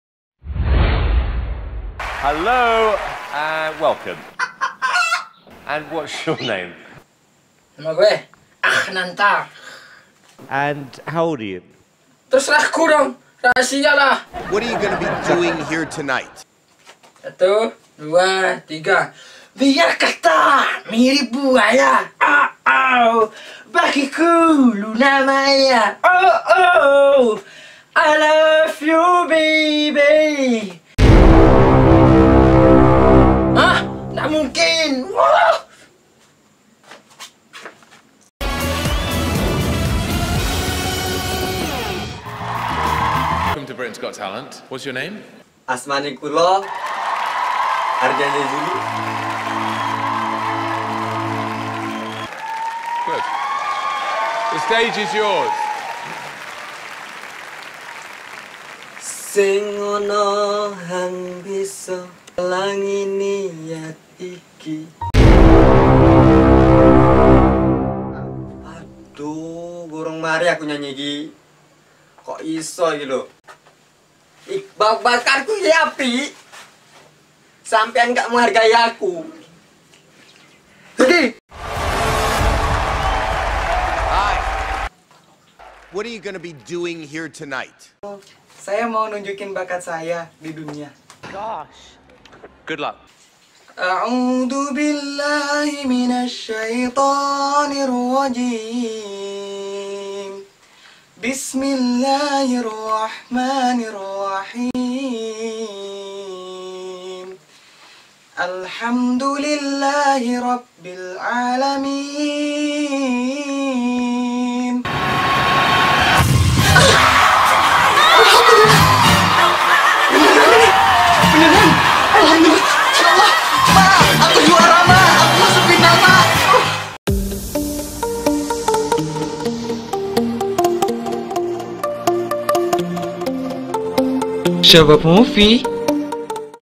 Mengejutkan!! Baca Al-Fatihah Dapat Gold Buzzer
Hello everybody!!

bantu saya mencapai 200 "subsriber" agar saya semangat membuat video-video yang terbaru

Tau acara besar inikna?? nah gimana nih kalau pesertanya membaca Al-Qur'an depan juri??

yuk Share jika bermanfaat